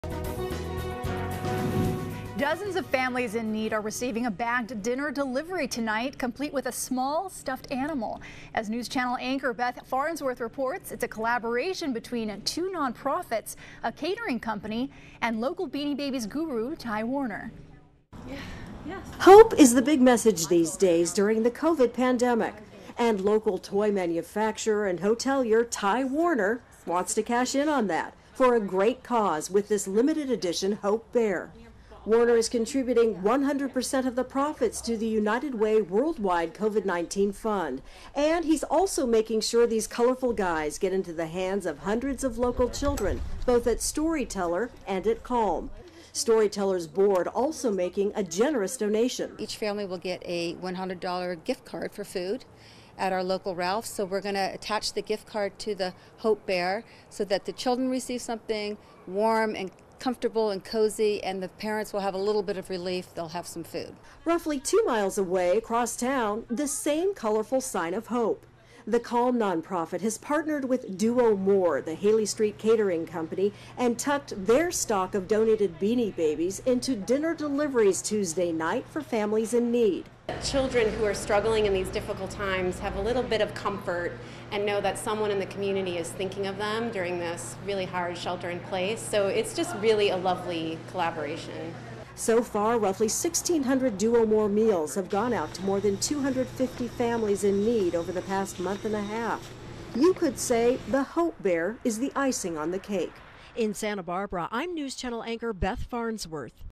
Ty Warner's newest Beanie Baby brings hope to children recovering from homelessness, trauma ...
Ty Warner's newest Beanie Baby brings hope to children recovering from homelessness, trauma during pandemic